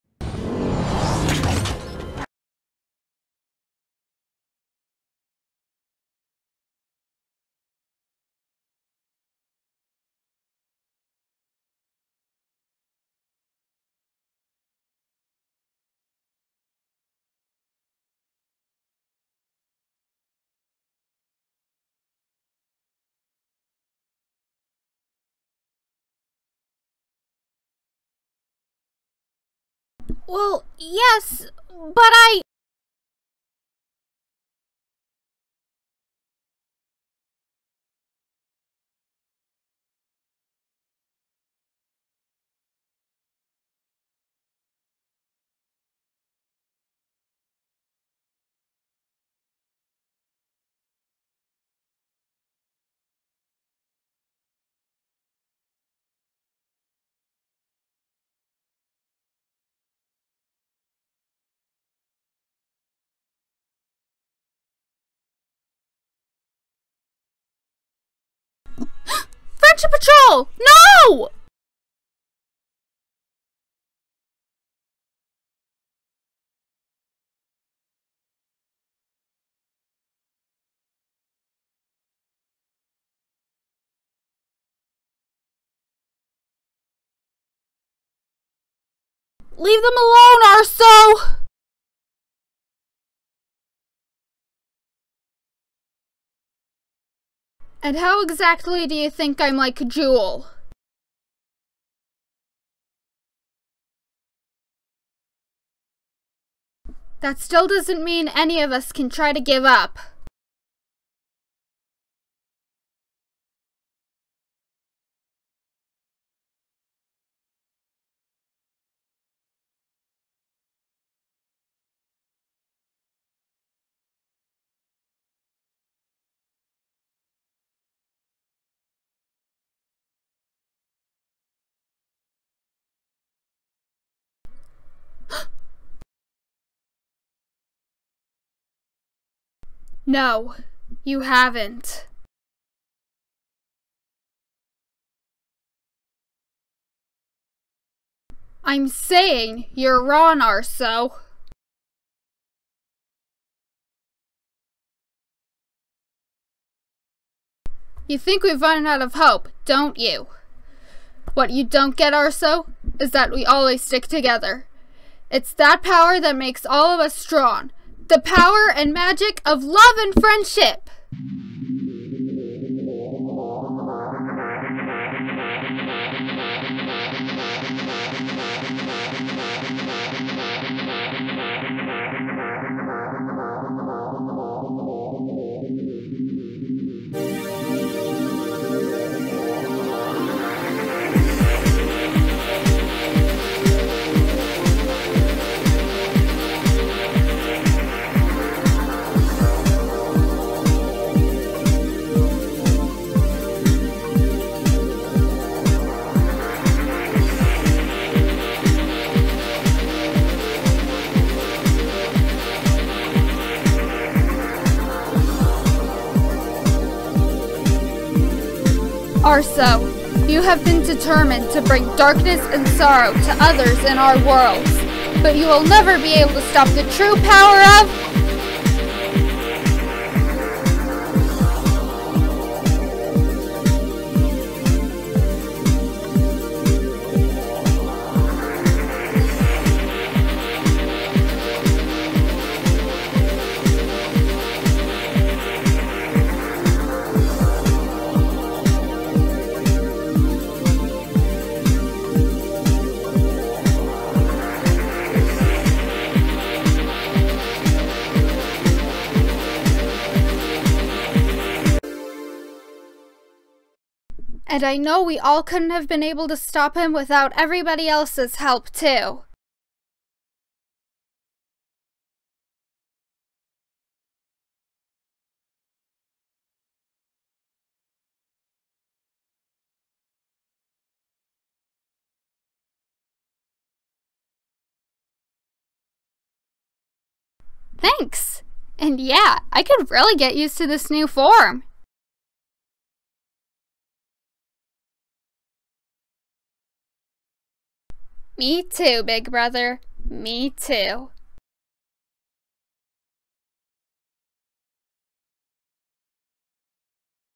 Adventures of Tirzah: A Cetsil Christmas scene - Arso's defeat
A scene where I finally get my Winter Magic of the Elements form and we all defeat Arso. Hope you guys enjoy!

Skylanders/Spyro owned by Activision
Ratchet and Clank owned by Sony
Wild Kratts and Odd Squad owned by PBS
Pym owned by Marvel
Ladybug owned by Zag Toon and Method Animation
Leonardo owned by Nickelodeon
Rainbow Dash owned by Hasbro
Tirzah/me, Striker, Transformation Theme, and Magic of the Elements Friendship Patrol (except Ratchet) owned by me